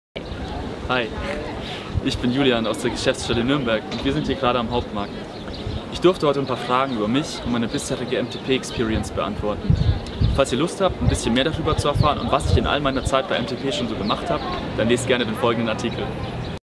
Verantwortung übernehmen fühlt sich großartig an!
Du hast Lust, mehr darüber zu erfahren, was MTP dir eigentlich bietet? Und bist gespannt in wieweit Julian aus der GS Nürnberg das bereits umgesetzt hat? Dann lies den folgenden Beitrag 0in unserem Magazin! 💌

Lasst uns gerne in den Kommentaren wissen, wie ihr das Interview findet. Danke für deine Zeit Julian